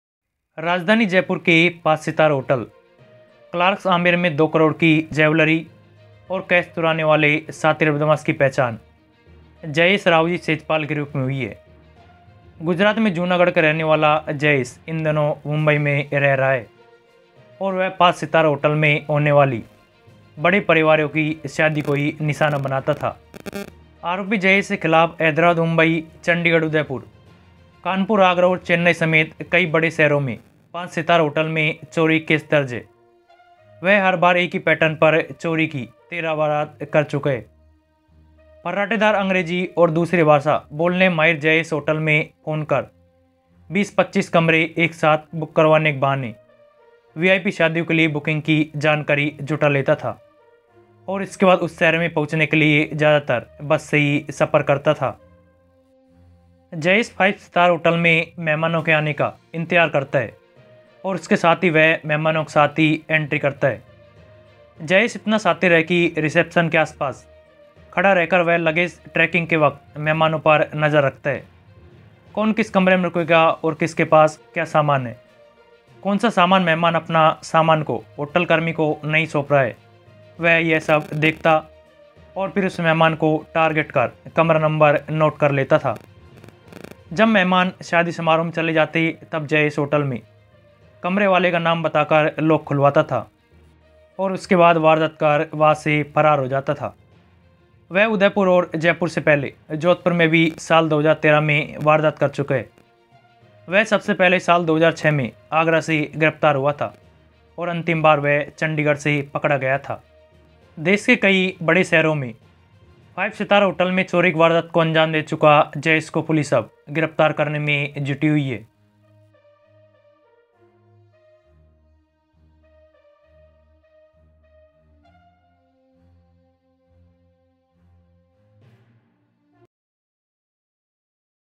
Jaipur में Clarks Hotal में हुई 2 करोड़ की चोरी करने वाला शख्त गुजरात का निकला पहले भी कर चुका है चोर
Jaipur Clarks Hotal में हुई 2 करोड़ की चोरी करने वाला शख्त गुजरात का निकला पहले भी कर चुका है चोरी

Jaipur News Clarks Amer Hotal Thief News Latest Jaipur Police News rajasthan news today Jaipur latest news